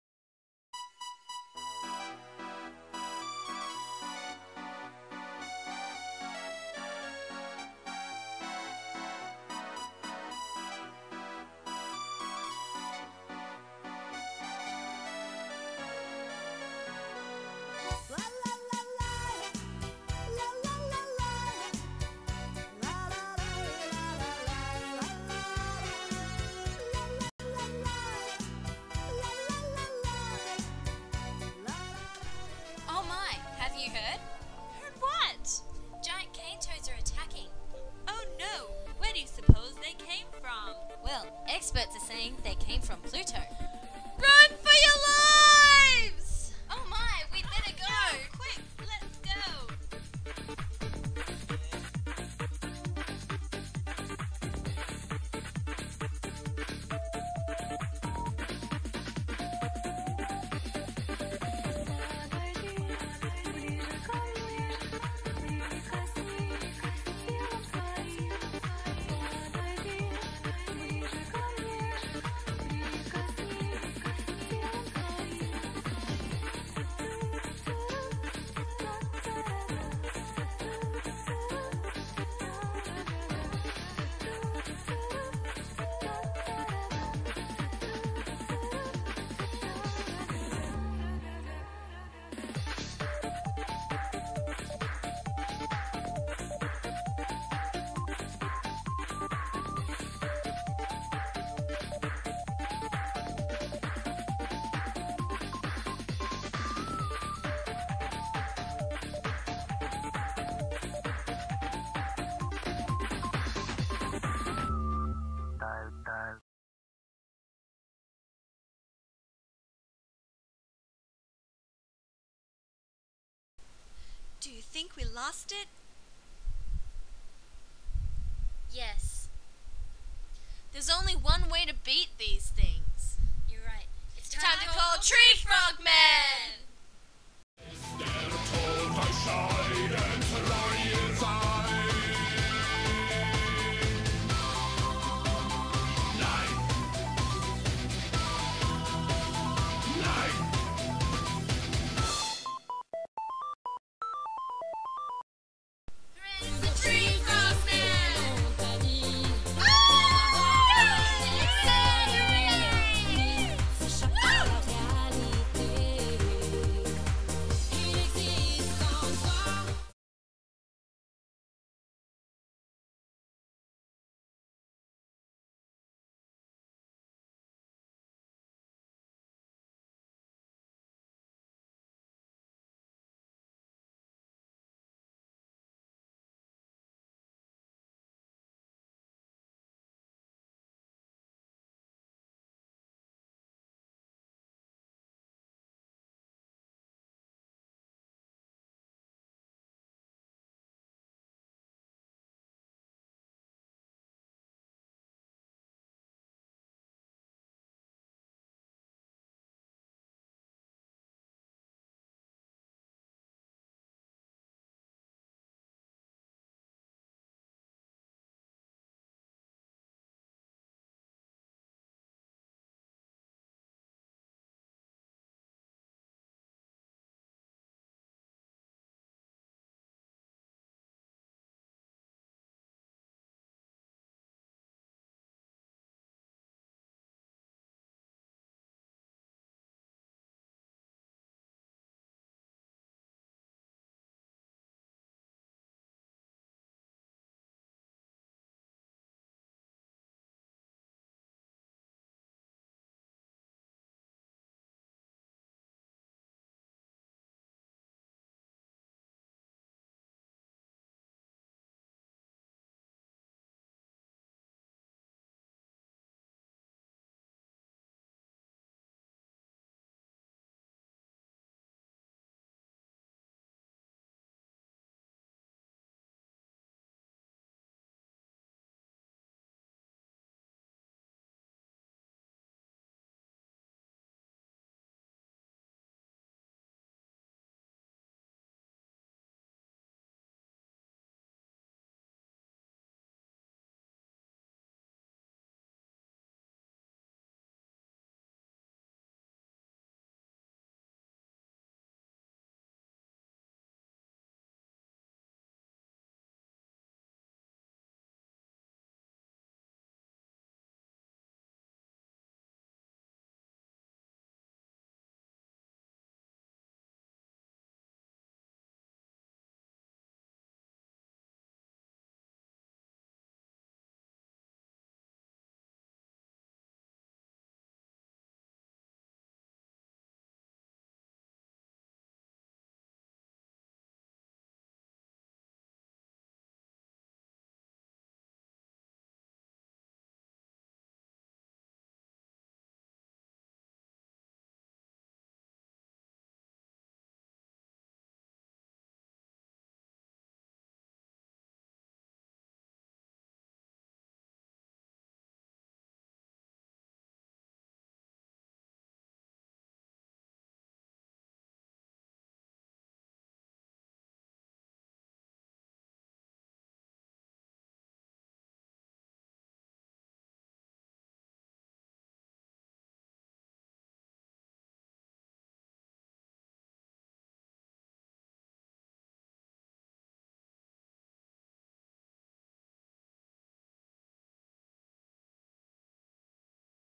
Attack of The Killer Cane Toads
An SBS spoof my friends and I made one very late night. Featuring Alysha, Jackie, and Leanne as part of a larger video made entitled "Res Angels" (2005).